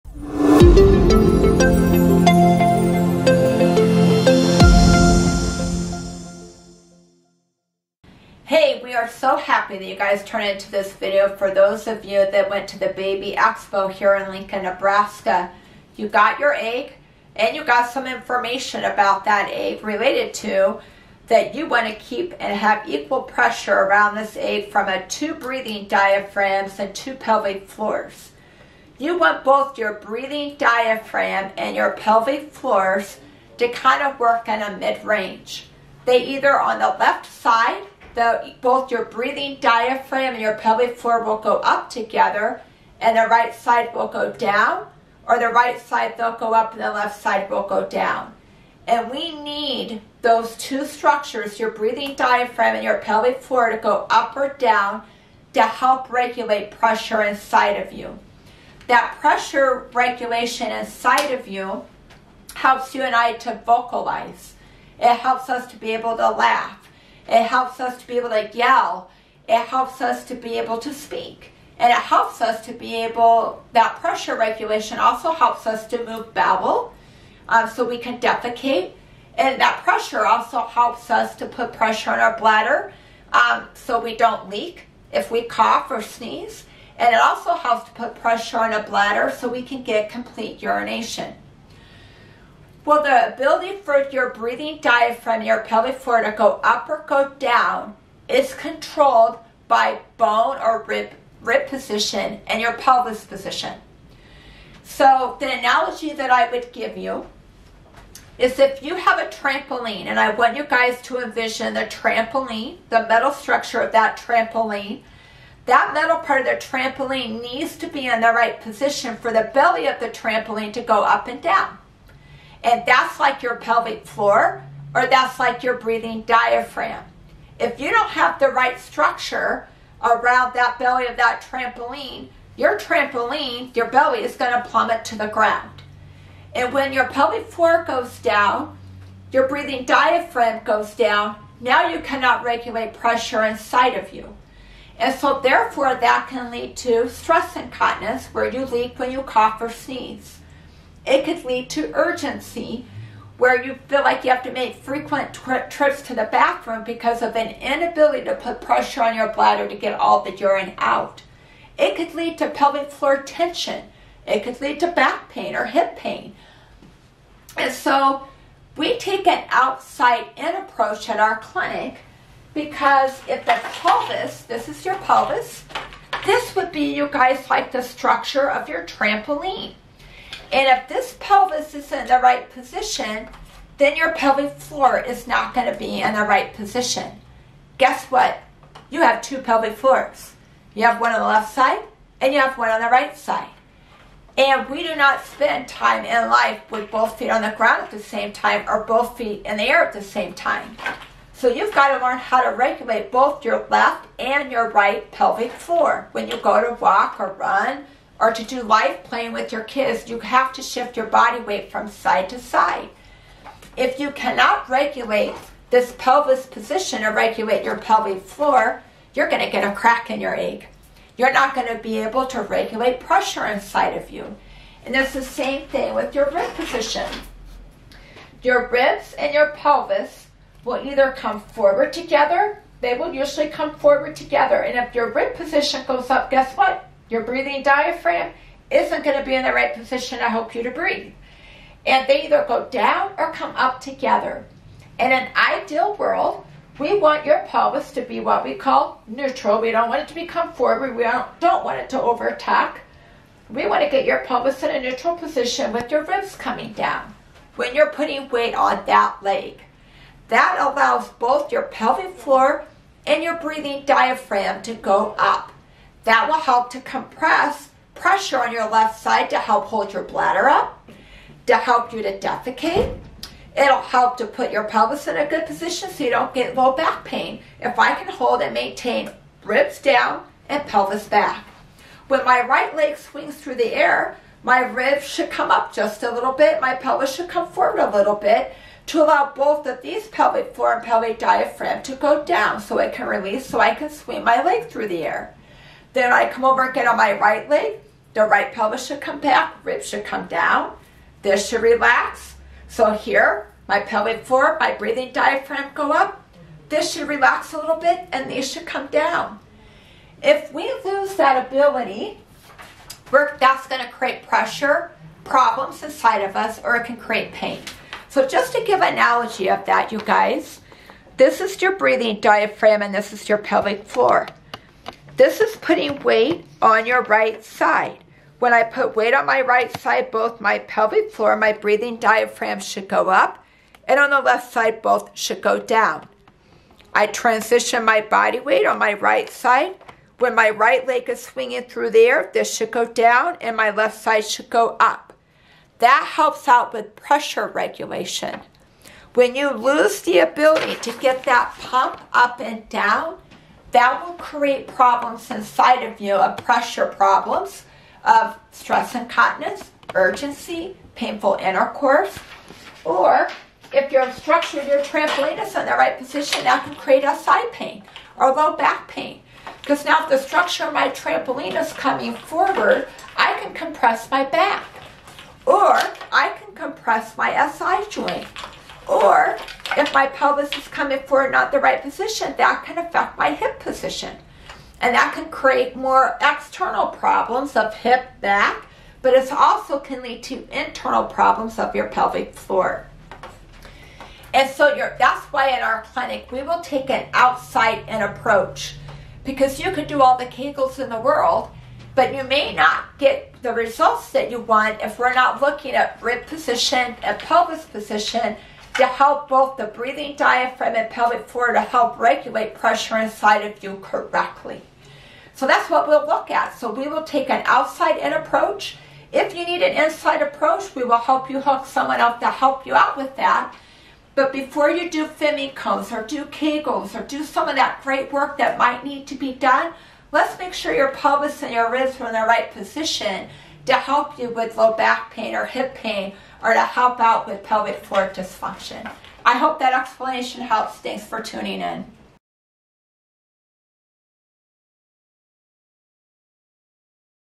Babe Lincoln: Introduction to a PRI approach to management of Pelvis and postural issues
An egg has a dome on the top and a dome on the bottom and when it is in the right position is a very strong structure.  Your pelvic floor muscles and breathing diaphragm muscles are the exact same thing.  When they are in the right position, as placed there by your pelvis and rib position, they are strong and can regulate your internal pressure.  If they are in an inappropriate position or the right and left sides do not work together you will have issues with regulating internal pressure which leads to incontinence, pelvic floor dysfunction, and potentially breathing or postural issues. The inability to use your pelvic floor diaphragm and respiratory diaphragm together will also lead to potential back, hip, SI joint, or other postural related issues.  At the Hruska Clinic we take an outside in approach to pelvic floor dysfunction by ensuring your egg (the pelvis floor and breathing diaphragm muscles) are in the correct position to work without needing to do uncomfortable or awkward internal assessment or treatment.   Listen to Lori describe this approach.  For more information on Postural Restoration or the PRI science